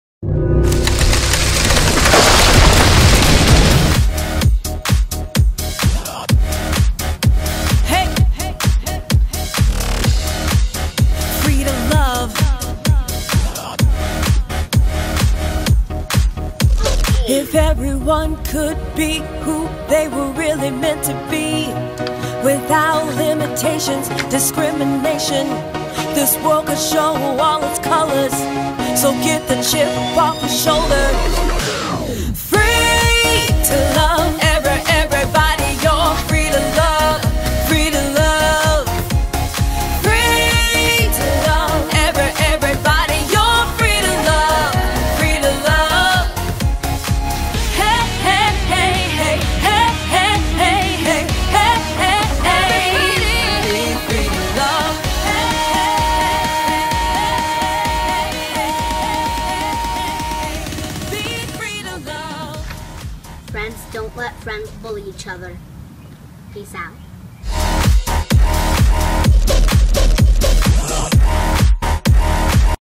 Jason Drucker Stands Up to Bullying
Youth Advocate Jason Drucker from Nickelodeon's 'Every Witch Way' is standing up to bullying for BullyingPreventionMonth and you can, too. Join Jason and Free2Luv.org, TakeThePledge and let your voice be heard!

Take a pic, send it in and share to spread the word